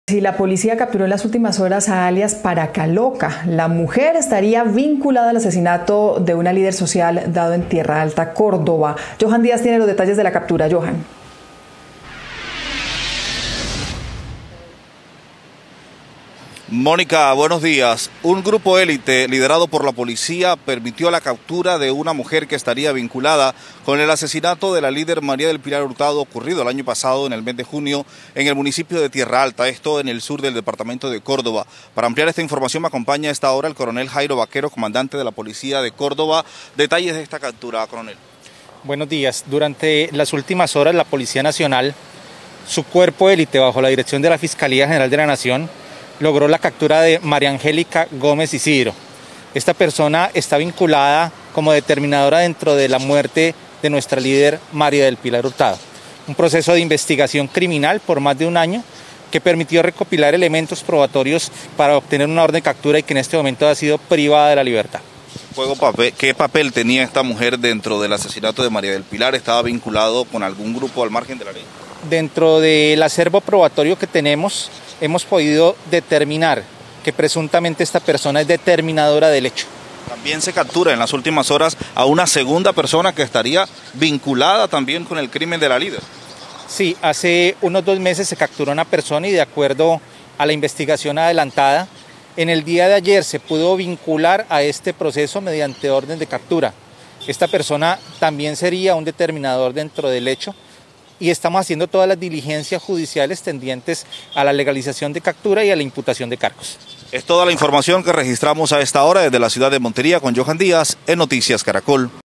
Cayó ‘Paraca loca’, vinculada con el doloroso asesinato de la líder María del Pilar Hurtado
Un año después del infame crimen de la mujer frente a su pequeño hijo, en Tierralta, Córdoba, la Policía detuvo a la presunta determinadora del hecho.